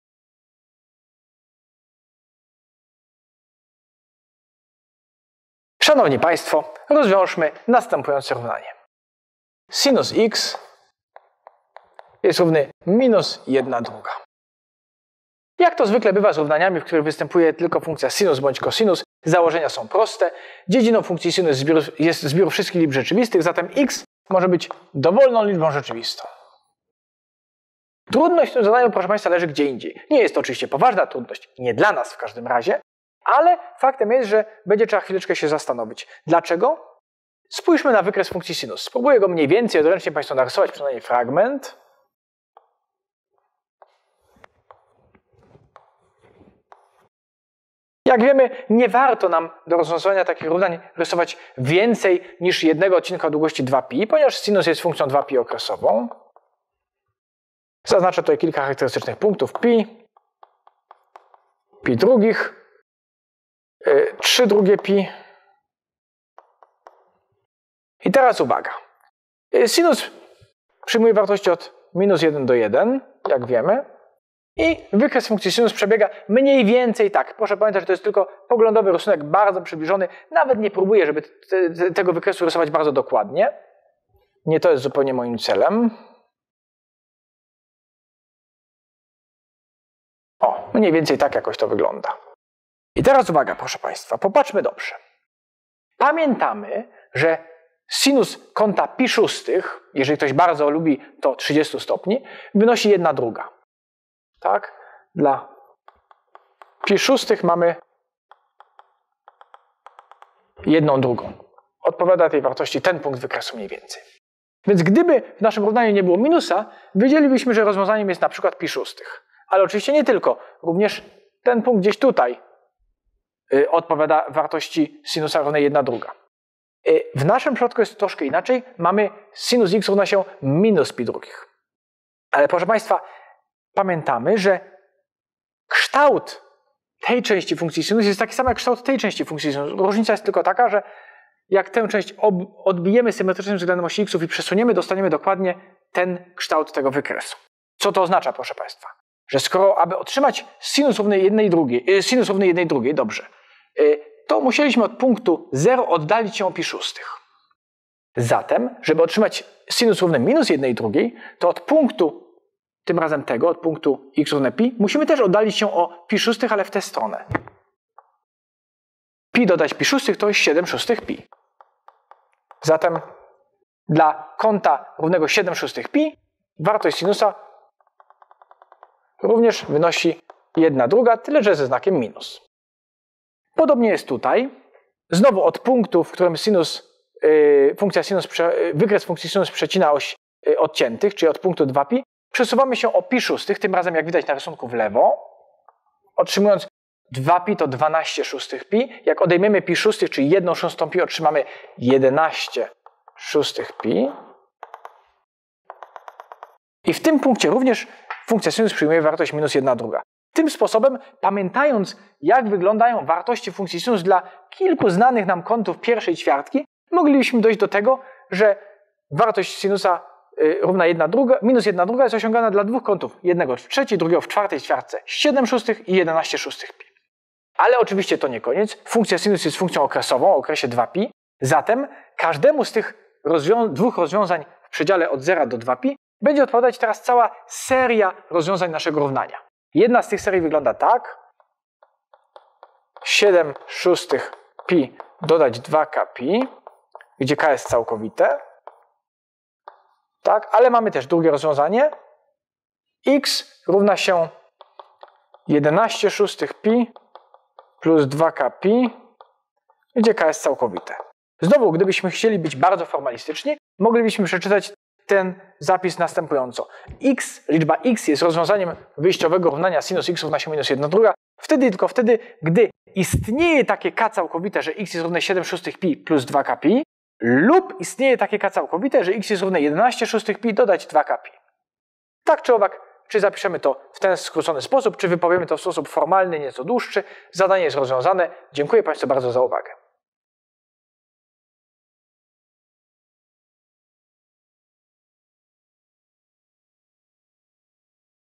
Równania i nierówności trygonometryczne - zadanie 1d
Podczas filmiku rozwiązujemy równość trygonometryczną.